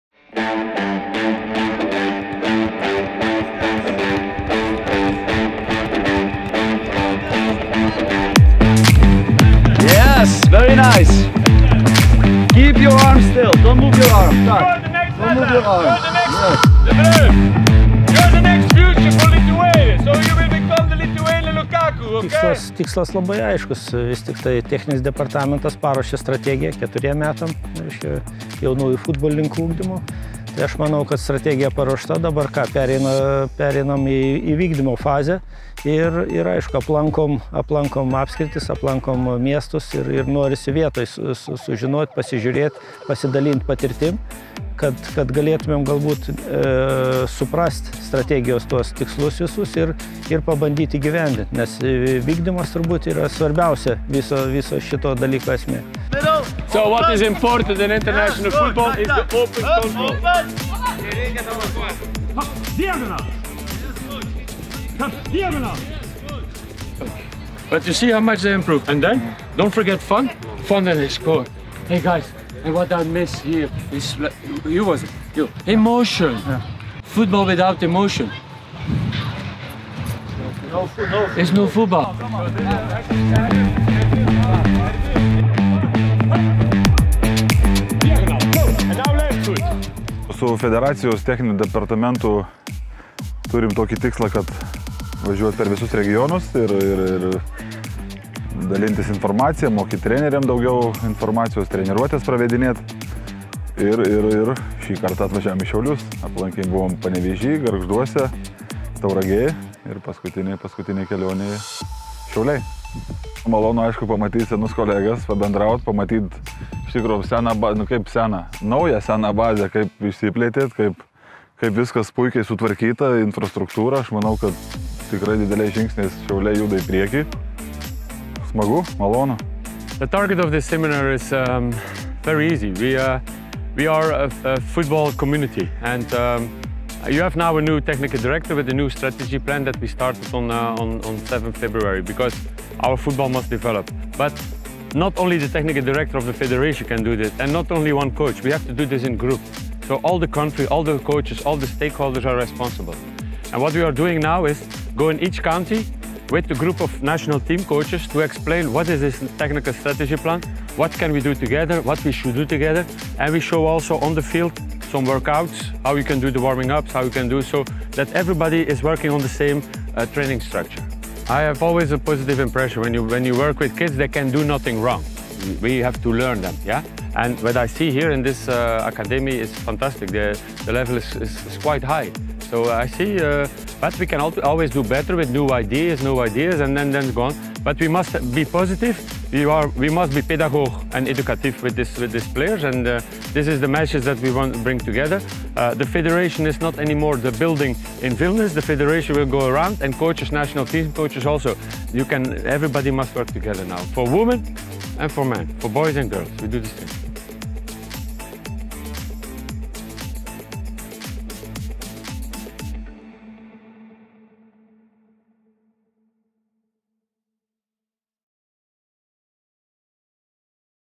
Futbolo akademija „Šiauliai“
Čia lankėsi LFF Techninis departamentas, kuris su vietos treneriais bei jaunaisiai žaidėjais dalinosi savo patirtimi! 👏
Patarimus dalino Lietuvos 🇱🇹 nacionalinės rinktinės vyriausiasis treneris Valdas Urbonas, LFF Techninis direktorius Patrick De Wilde, buvęs FA „Šiauliai“, o dabar Lietuvos jaunimo rinktinių vartininkų treneris Audrius Paškevičius bei kiti jų kolegos! Video reportaže 🎥 išgirsite ir pastarųjų mintis apie praėjusią dieną!